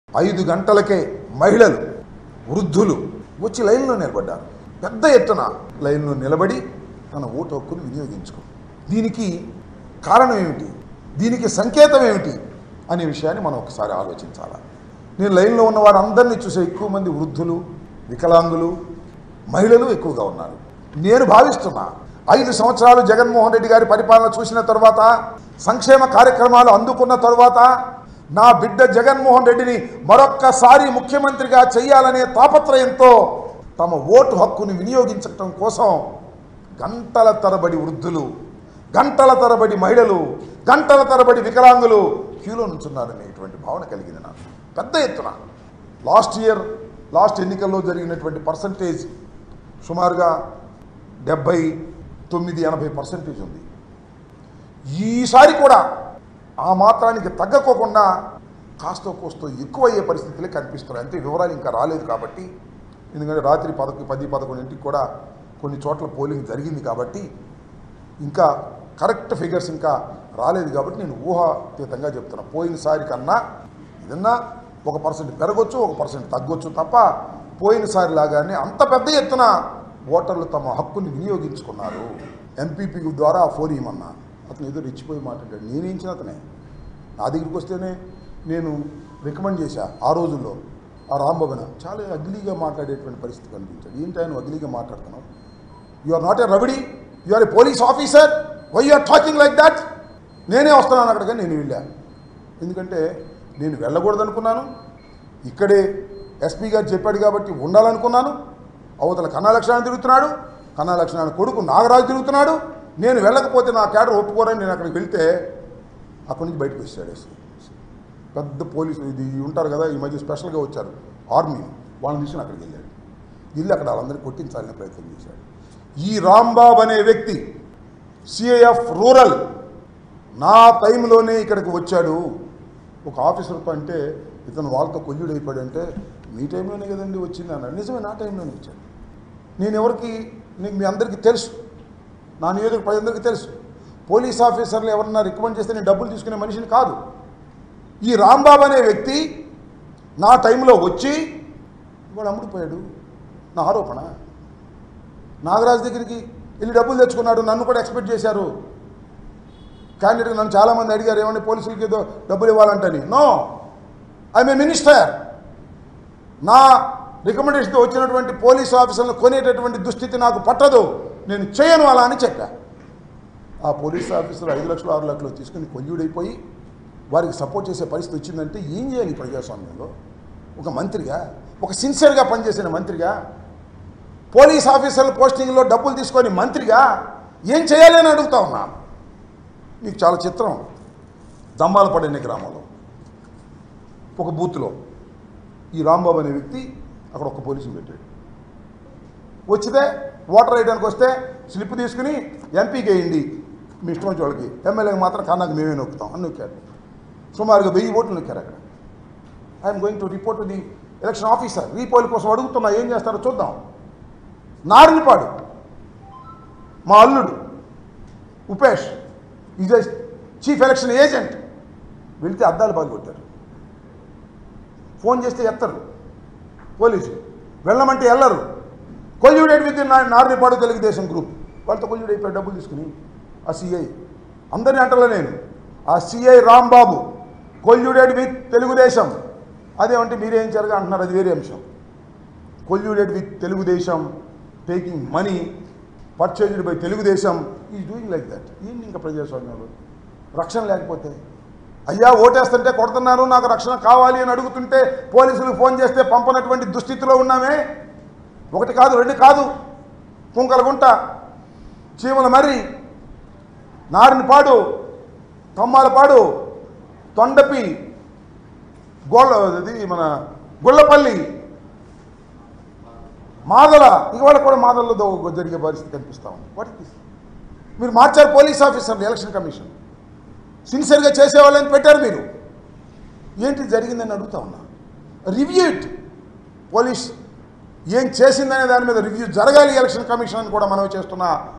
ఆ గ్రామంలో రీపోలింగ్ నిర్వహించాలి.. అంబటి రాంబాబు | Ambati Rambabu Press Meet
Watch :: ఆ గ్రామంలో రీపోలింగ్ నిర్వహించాలి.. అంబటి రాంబాబు | Ambati Rambabu Press Meet | @viswabharathtv